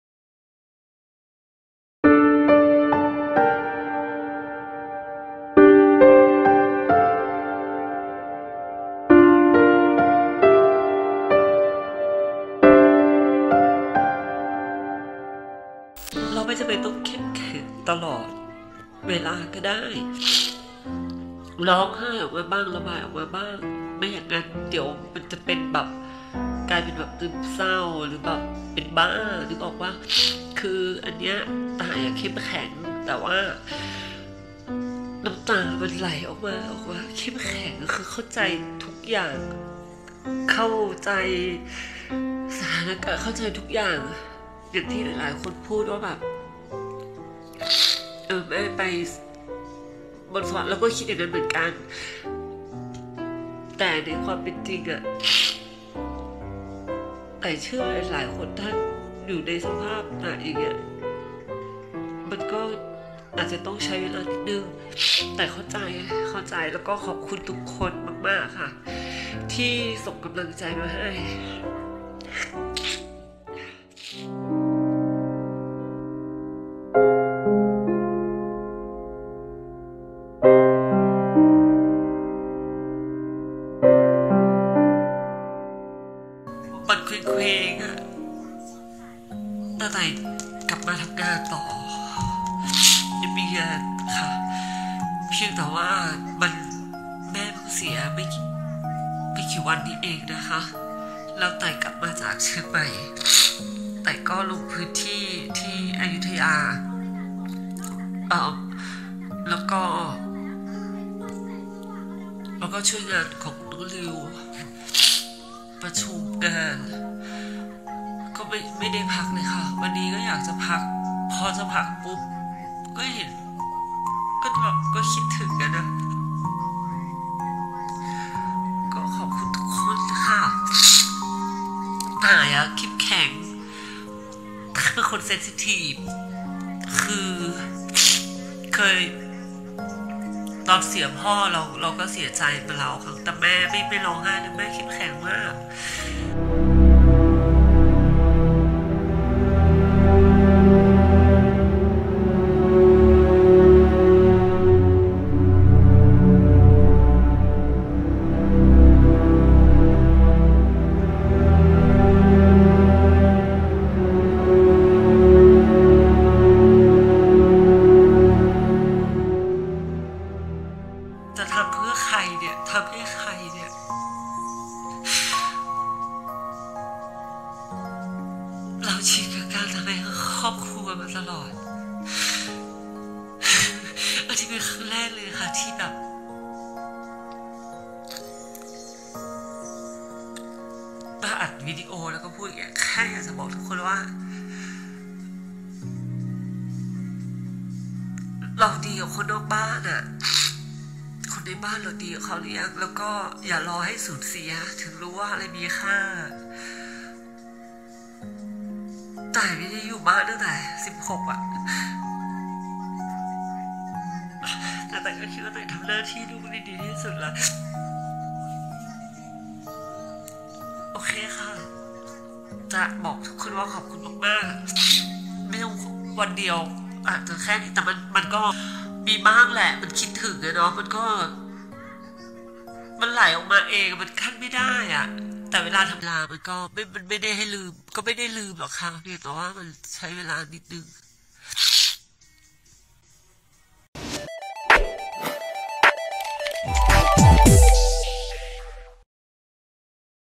ต่าย สายธาร ร่ำไห้ คิดถึงคุณแม่ แค่เห็นรูปน้ำตาก็ไหล
แค่เห็นรูปน้ำตาก็ไหล ต่ายสายธาร ร่ำไห้ ระบายความในใจ คิดถึงคุณแม่ที่จากไป เชื่อทำหน้าที่ลูกดีที่สุดแล้ว